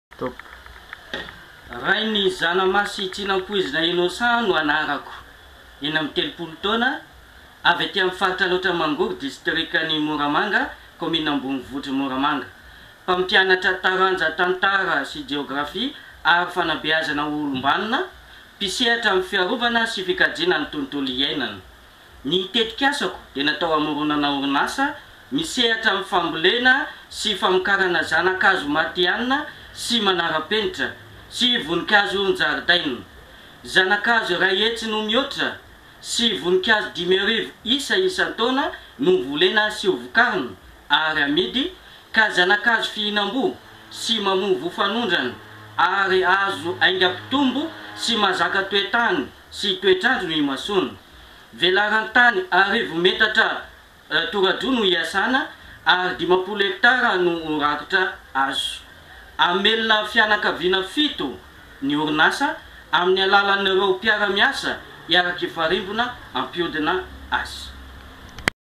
FAMBOLENA SY FAMORONANA ZANA-KAZO SY VONINKAZO MATIHANINA SY MANARA-PENITRA
Ce projet consiste à créer une entreprise spécialisée dans la plantation et la production de plantes et fleurs. Il met l'accent sur la conservation de l'environnement et est conçu pour améliorer les arbres, pour ceux qui ont l'habitude de cultiver des arbres fruitiers et d'exporter des fruits. Une Superficie de 1 000 m2 de terrain produira 100 000 arbres et 5 000 fleurs par an. Le prix est censé être abordable pour le grand public tout en assurant le commerce. Les entreprises emploieront des personnes et le taux de chômage diminuera.